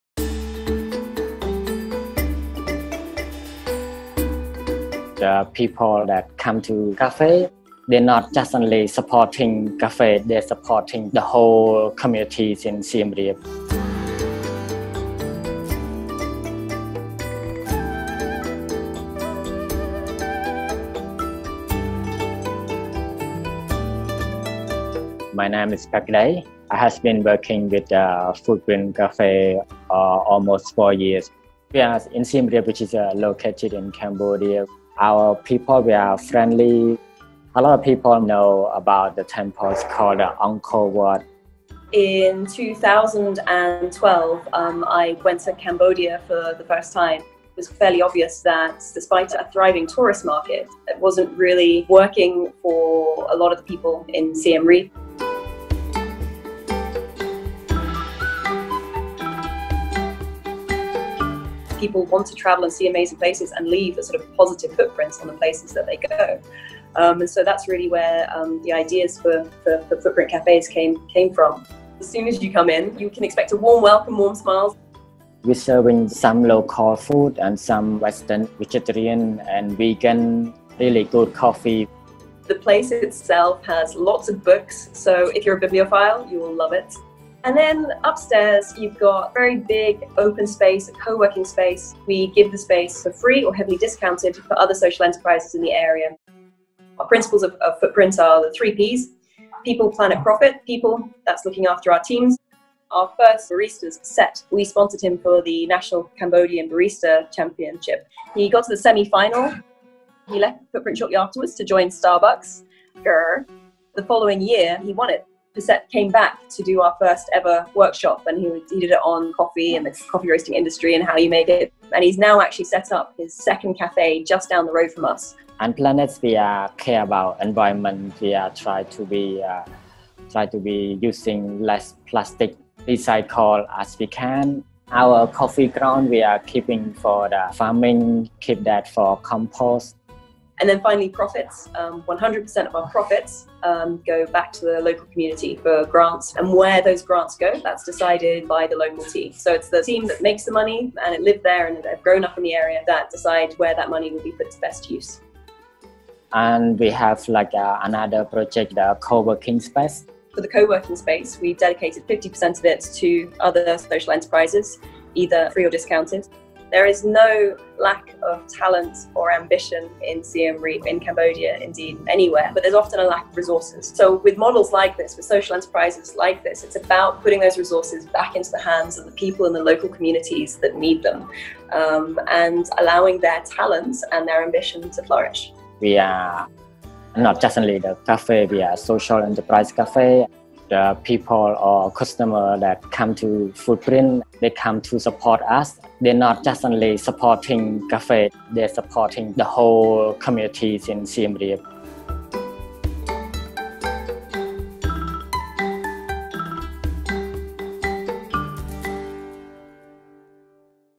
Footprint Cafés: Best in Travel 2021 Community Small Business
Tucked away in the Cambodian city of Siem Reap is the Footprint Cafés, an eatery that provides Khmer dishes, Cambodian coffee, and a healthy dose of opportunity for several local businesses.

Download Lonely Planet’s Guides App for thousands of recommendations curated by our experts: